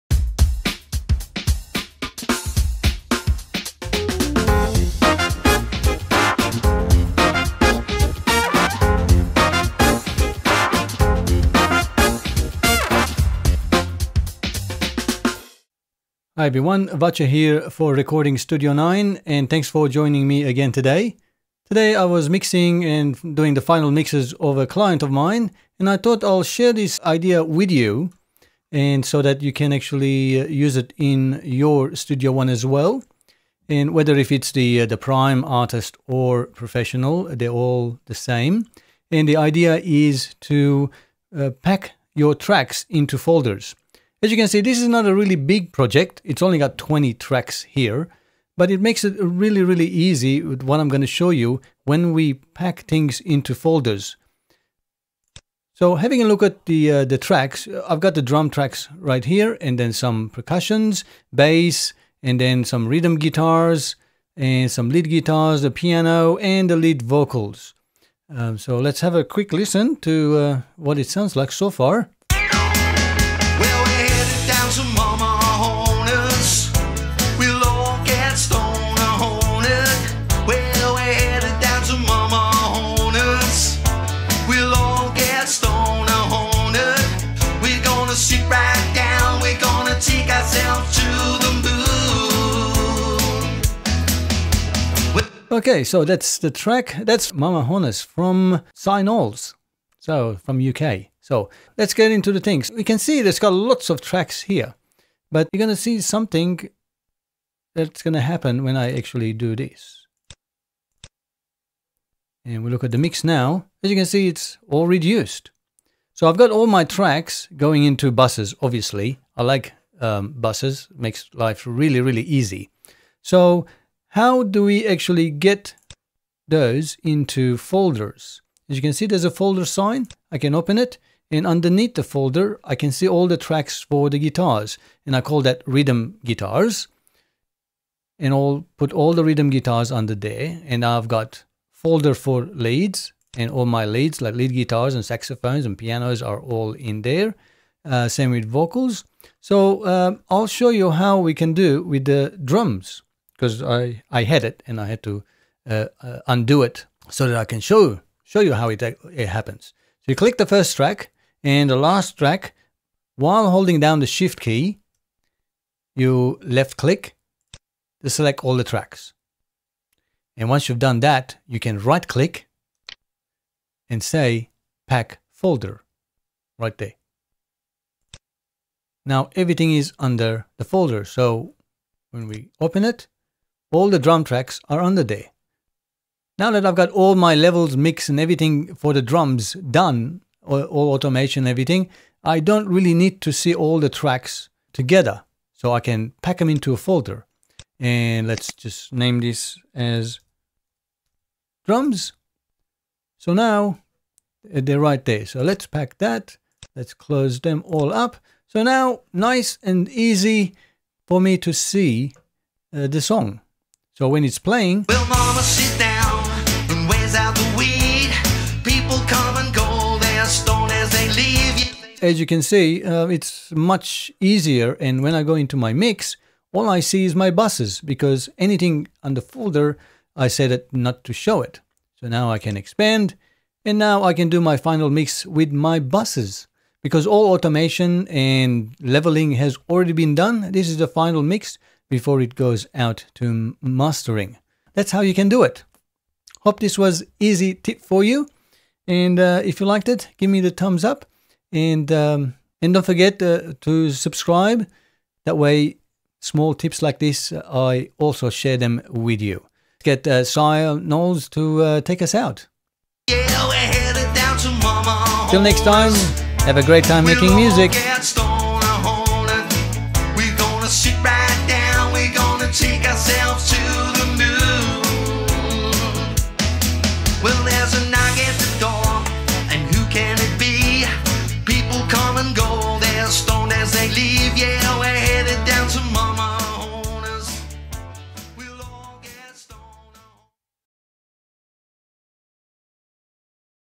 PreSonus Studio One V3 Track Pack Folder Option Quick Tip
If you have lots of tracks for your song project in Studio One V3, I'll show you how you can pack them into folders to maximize the available timeline and mixer space.